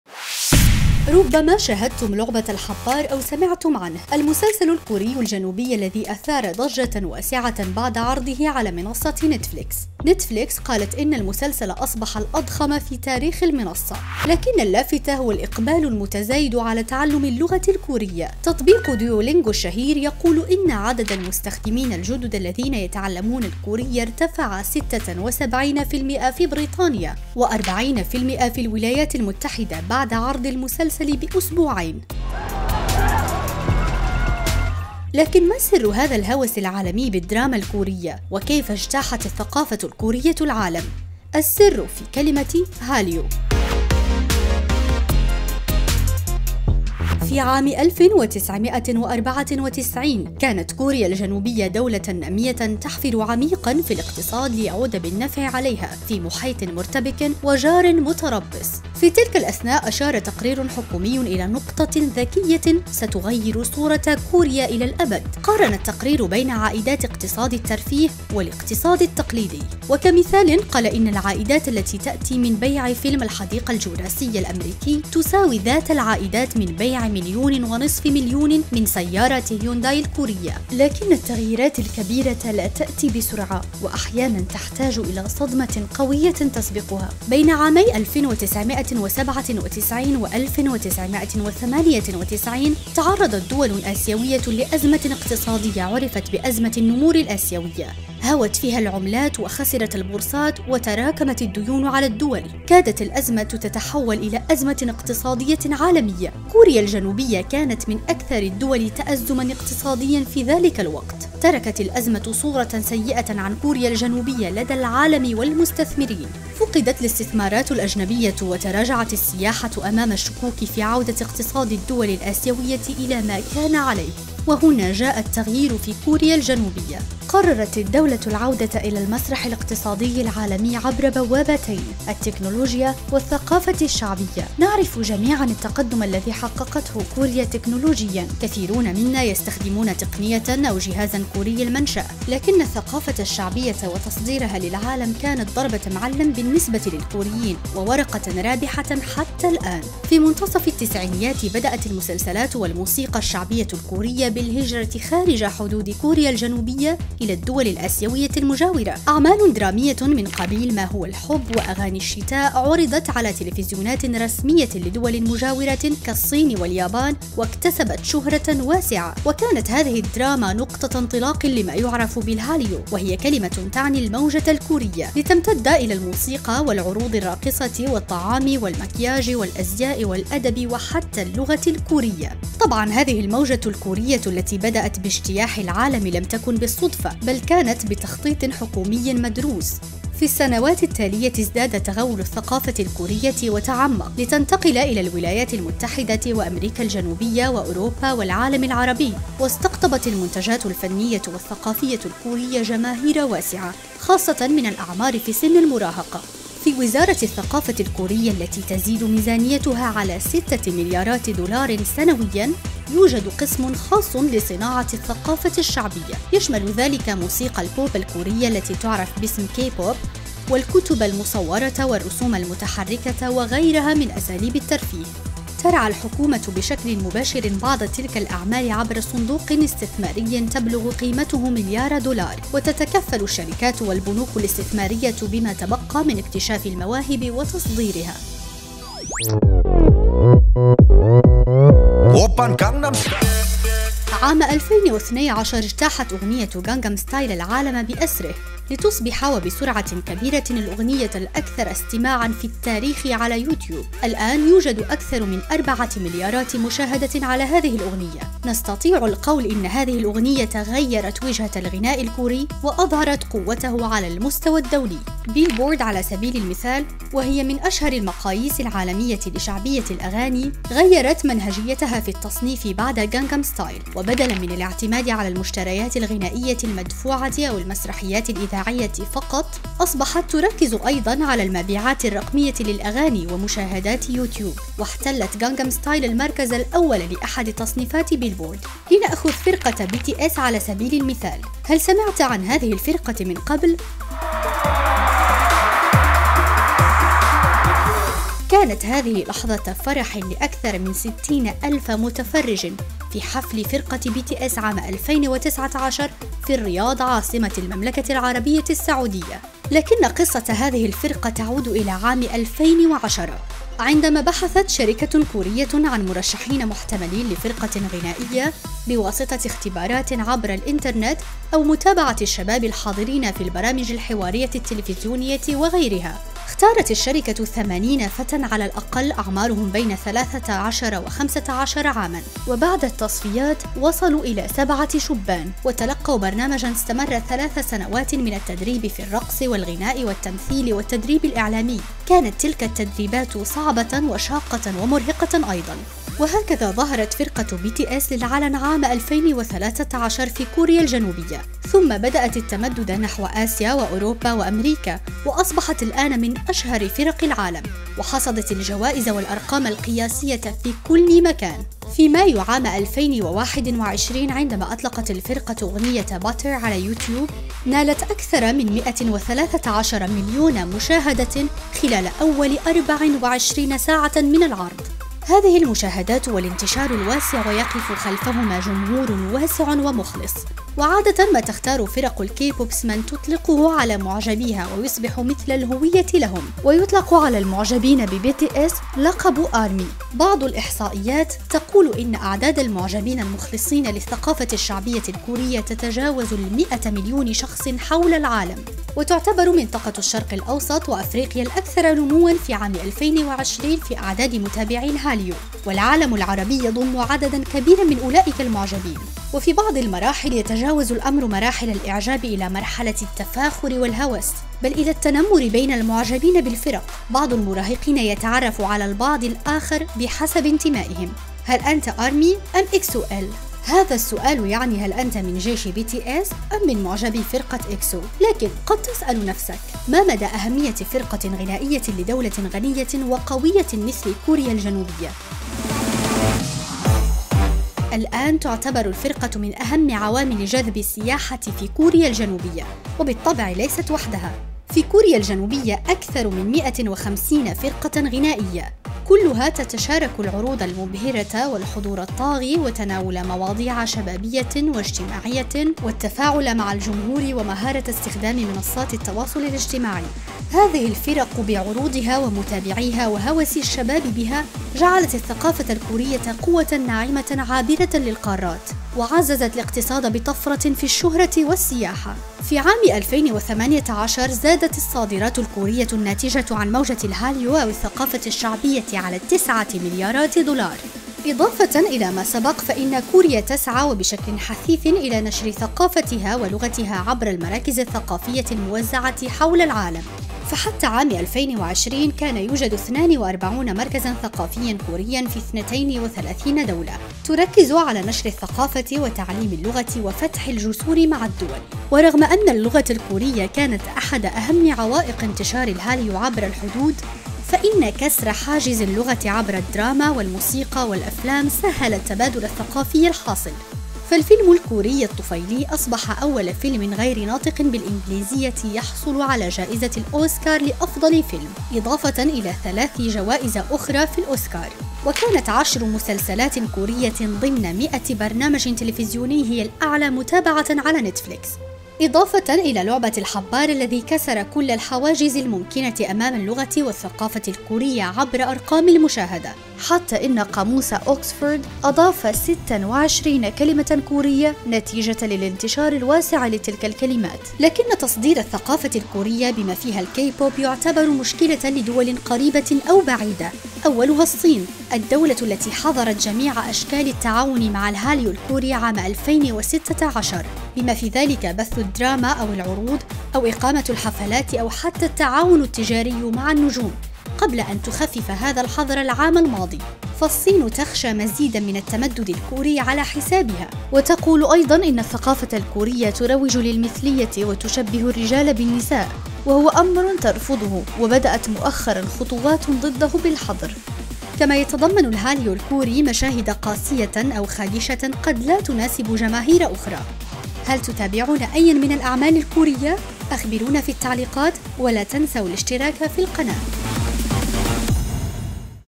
الموجة الكورية الجنوبية تجتاح العالم.. السر في كلمة "هاليو"
بعد سنوات من التخطيط والدعم الحكومي ونشاط الشركات الخاصة، الثقافة الشعبية الكورية الجنوبية تغزو العالم بالأفلام والمسلسلات والأغاني وغيرها، وأعداد المقبلين على هذه الثقافة في ازدياد.